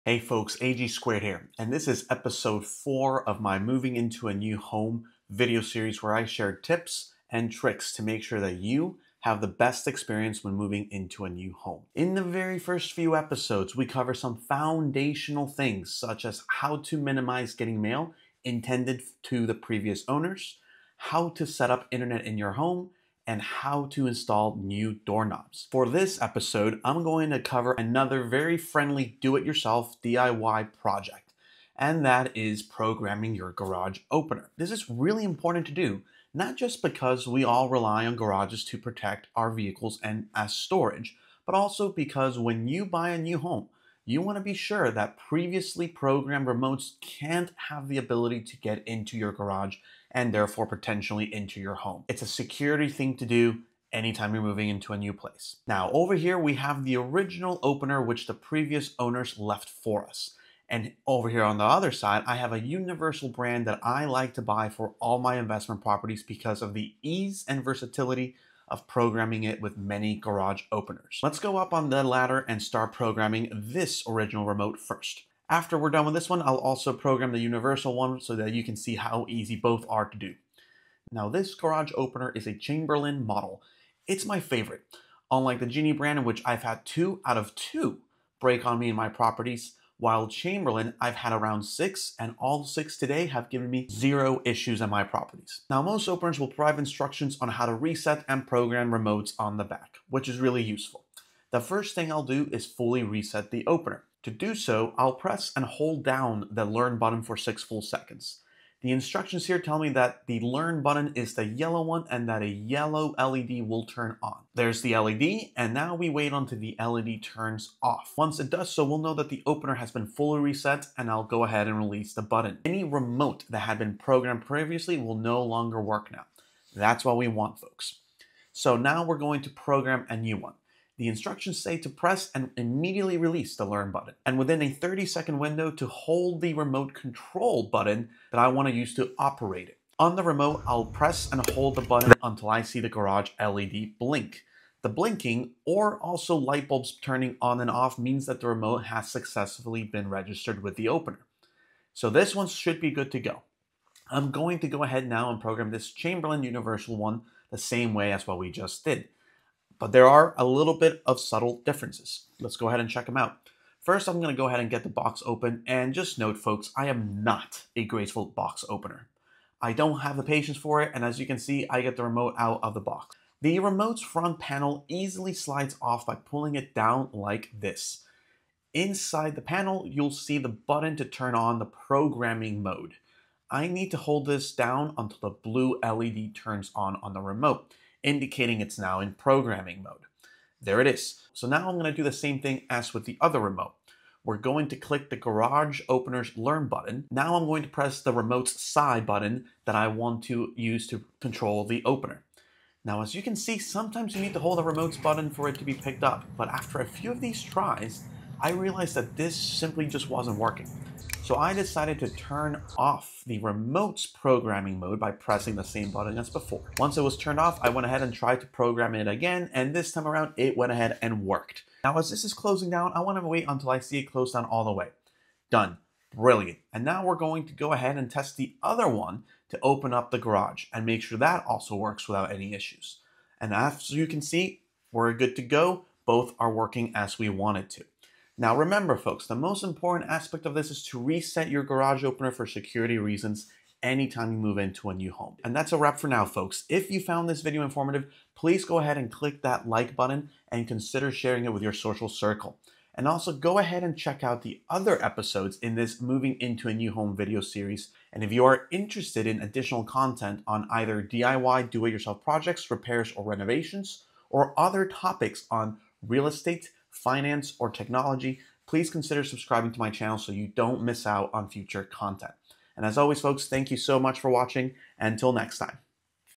New Home? Reset and Program Your Garage Door Opener
When moving into a new house, one of the first tasks should be reprogramming your garage door opener for security reasons. In this video, we’ll guide you through the steps to reset and reprogram both an original and a universal remote for your Chamberlain garage door opener. We’ll share why
the Chamberlain brand is my preferred choice over others and provide a hands-on demonstration to ensure your garage is safe and secure.

💡 Highlights:
• Resetting the opener and clearing previously programmed remotes
• Step-by-step instructions for programming original and universal remotes
• Practical tips for DIY enthusiasts

🔧 Gear Used:
• Original garage door opener remote
• Chamberlain universal remote

Chapters
0:00 Intro
0:35 Why to reset garage opener codes
1:35 How to reset the garage codes
2:08 How to program a new remote
2:37 How to program chamberlain universal opener